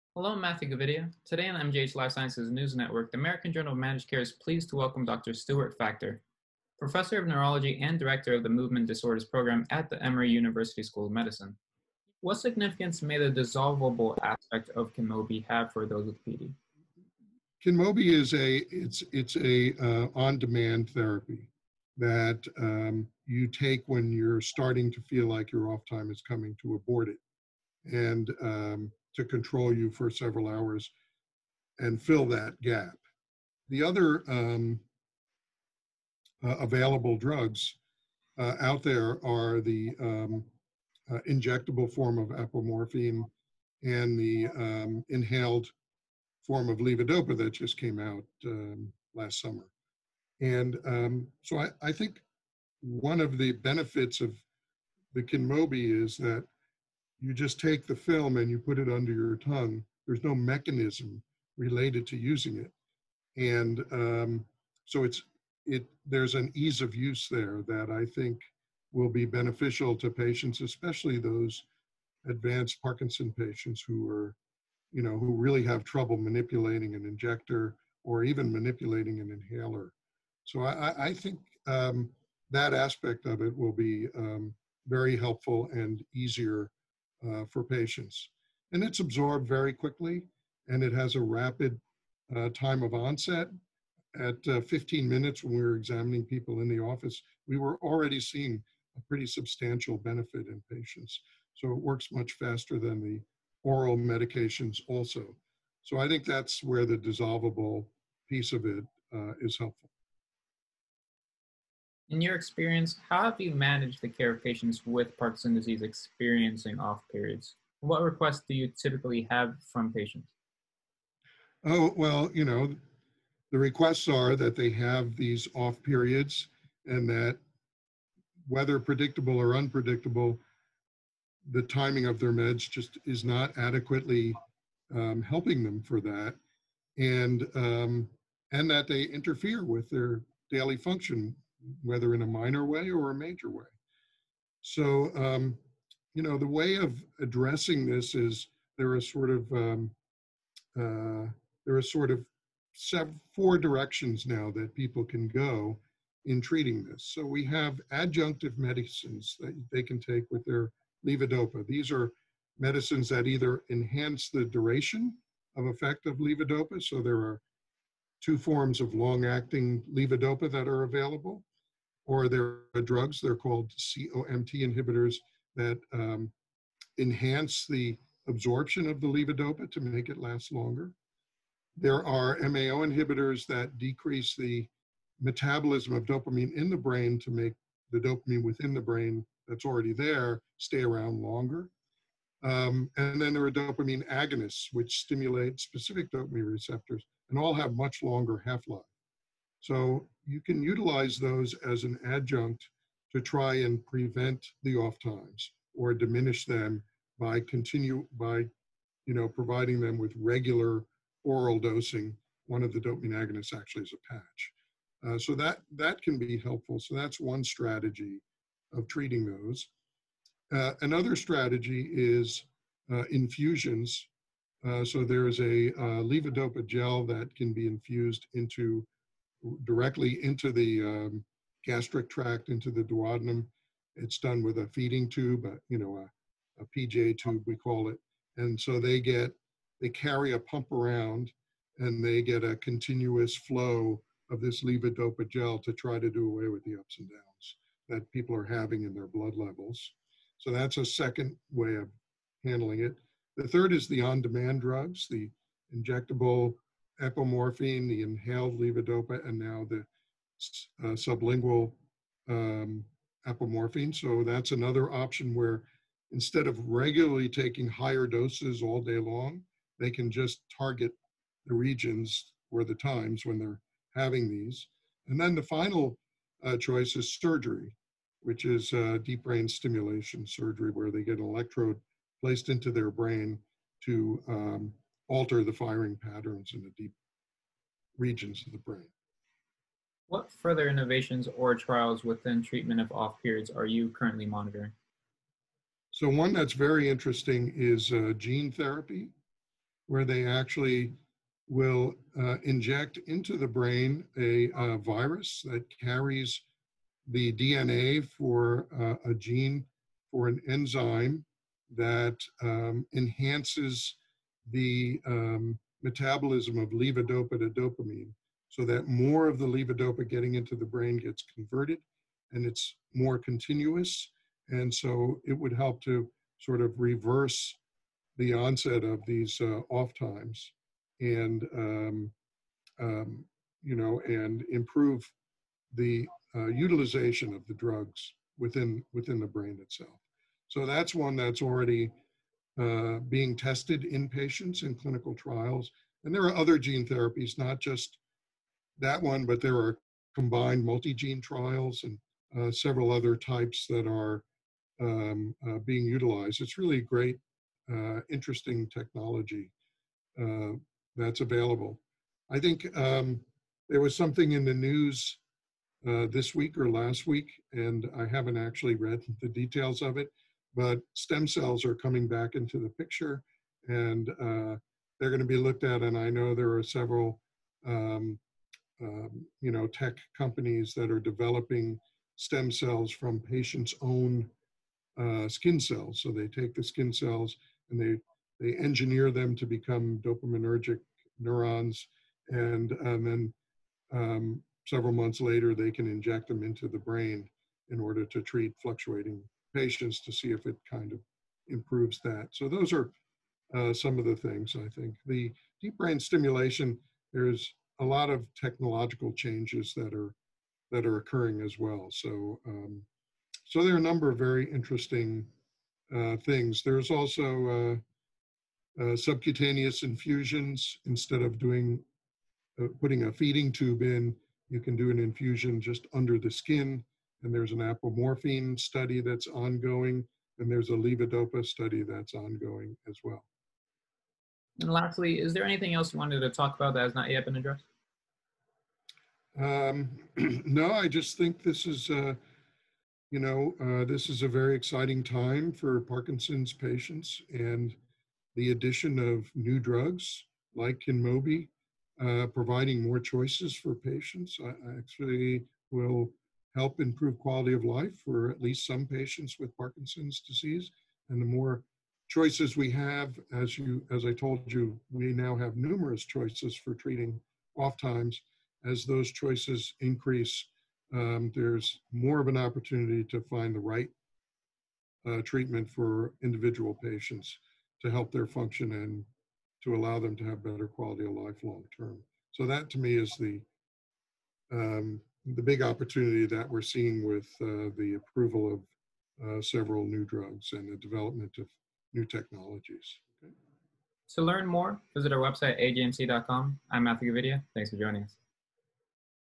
Dr Stewart Factor Discusses Management of OFF Periods, Treatment Innovations in Parkinson Disease
The addition of a discreet, under the tongue treatment of OFF periods in patients with Parkinson disease, adds to the growing market of innovations seeking to individualize patient care, said Stewart Factor, DO, professor of Neurology and director of the Movement Disorders Program at the Emory University School of Medicine.